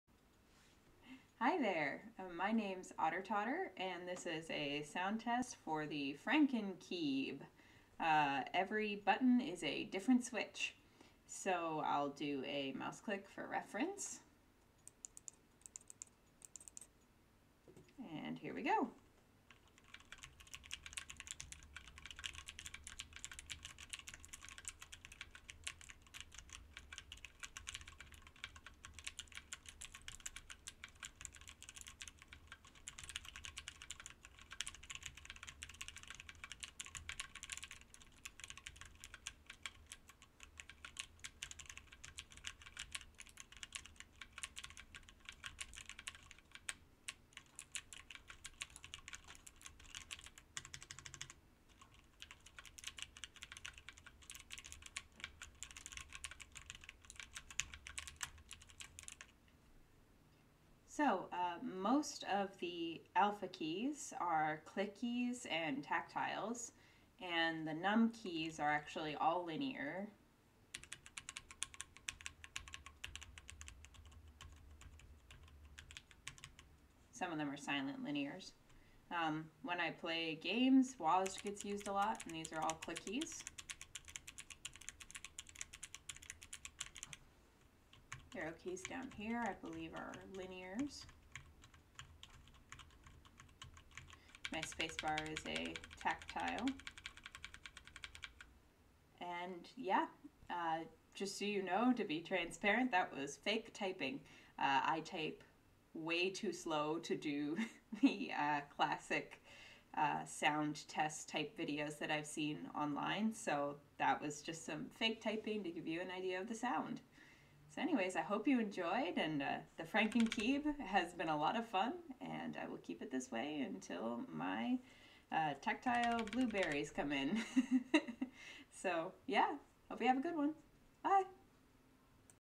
Franken Keeb Sound Test
This is a sound profile test for the Franken Keeb! The Franken Keeb is a mechanical keyboard that I put together with a different switch for every button.

I was NOT paid or given this product to promote it.